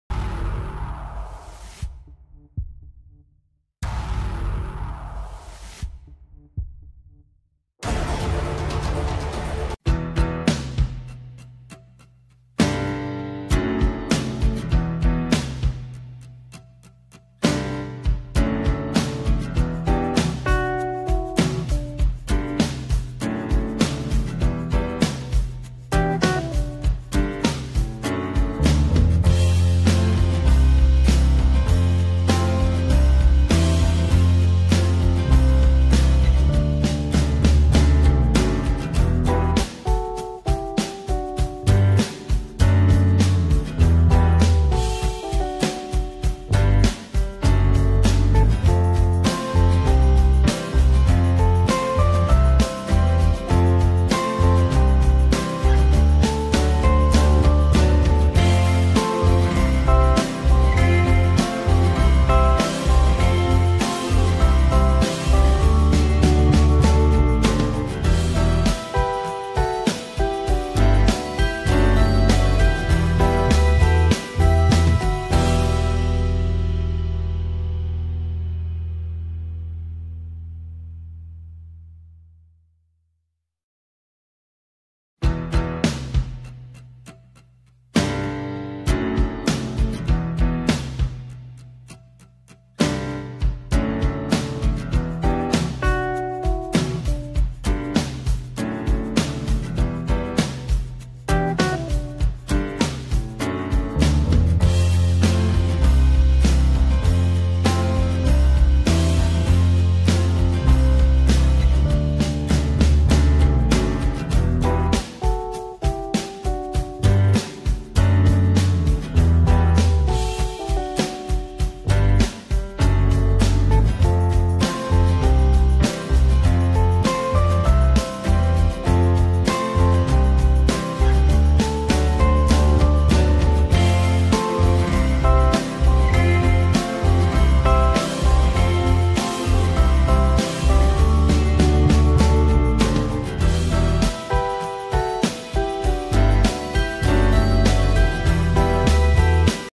一勺紅糖加點它頂過10個老軍醫，專治膝蓋，一次緩解，7天自如，記得存
日常生活中，我們的人體是很脆弱的，稍一不注意就會對我們的身體造成損傷。很多時候，我們會感覺膝蓋疼痛，而我們又不知道膝蓋痛是什麼原因。今天，就來瞭解一下導致膝關節疼痛的幾個因素。